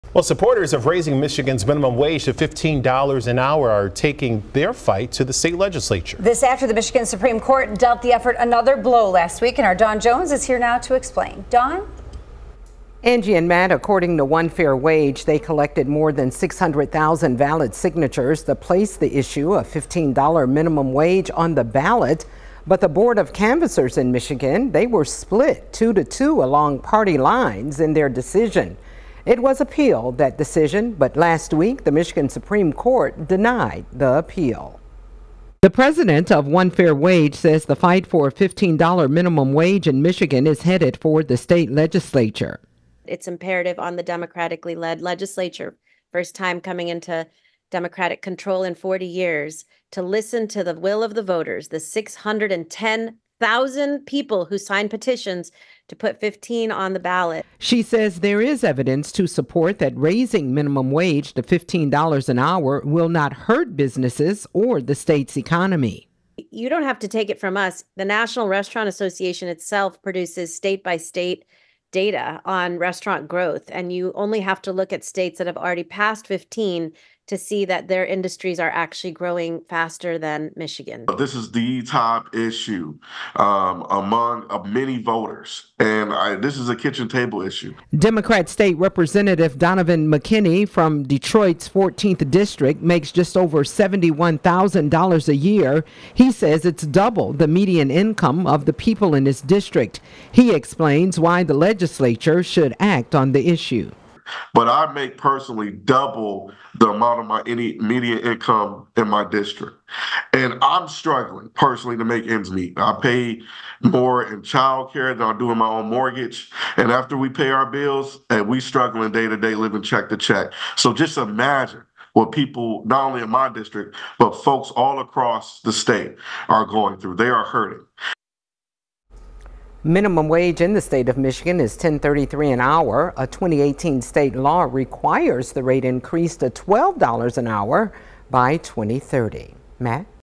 $15/hr minimum wage won't be on the Michigan ballots in 2024
One Fair Wage will continue to fight for $15 an hour by taking their case to the Michigan legislature.